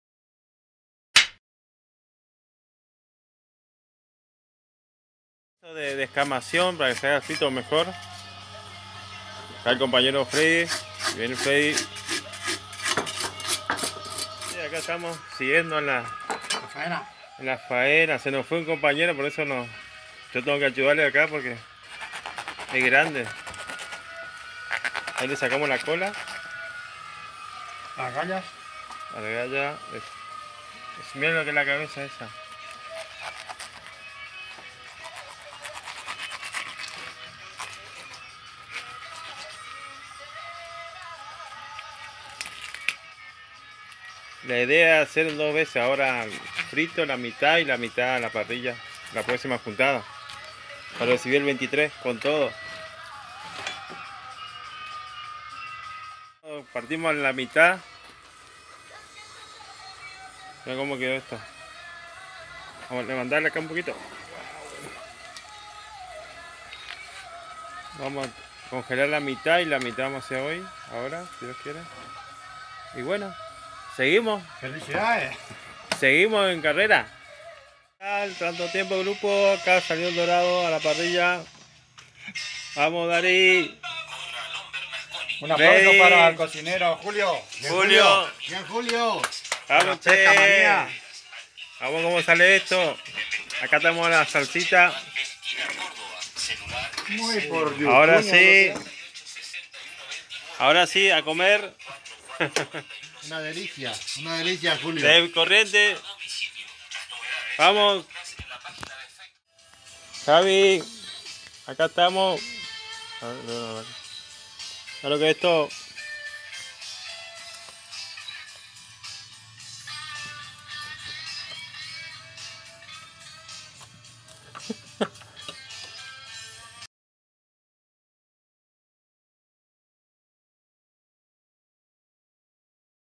Dorado a la parrilla, la banda de Pezcamania desde corrientes Empedrado. Gracias
Un Dorado a la parrilla realizado por la banda de Pezcamania de Corrientes Empedrado, julio y amigos, gracias por compartir con nosotros ese rico Dorado a la parrilla del Parana de Corrientes.